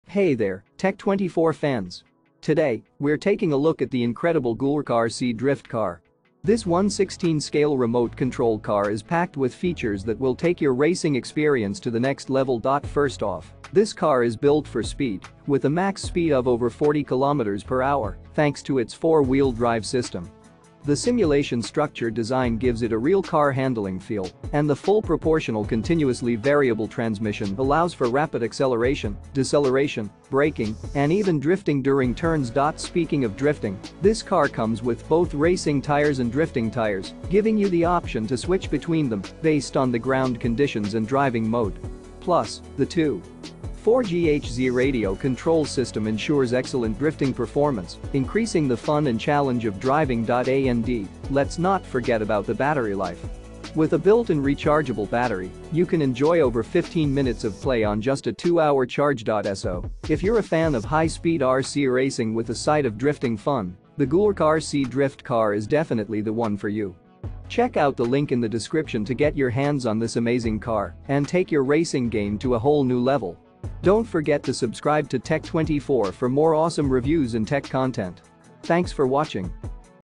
Buy GoolRC RC Drift Car, 1:16 Scale Remote Control Car, 4WD 40KM/H High Speed RC Racing Car, 2.4GHz
Buy and Review GoolRC RC Drift Car, 1:16 Scale Remote Control Car, 4WD 40KM/H High Speed RC Racing Car, 2.4GHz Electric Sport Driftings with Headlight, Drifting Tires and Racing Tires for Adults (Black) on AMAZON Shop

Experience the thrill of high-speed RC racing and drifting with the GoolRC RC Drift Car! Join us on Tech 24 as we take a detailed look at this incredible 1:16 scale remote control car that's designed for maximum performance. With a top speed of over 40km/h, four-wheel drive system, and simulation structure design, this car delivers a real car handling feel like no other. Plus, with the inclusion of both racing tires and drifting tires, you have the flexibility to adapt to different driving conditions and modes. The 2.4GHz radio control system ensures excellent drifting performance, adding a new level of challenge and excitement to your racing experience. And with a rechargeable battery that provides over 15 minutes of play on just a 2-hour charge, the fun never has to stop. Don't miss out on the opportunity to elevate your racing game with the GoolRC RC Drift Car.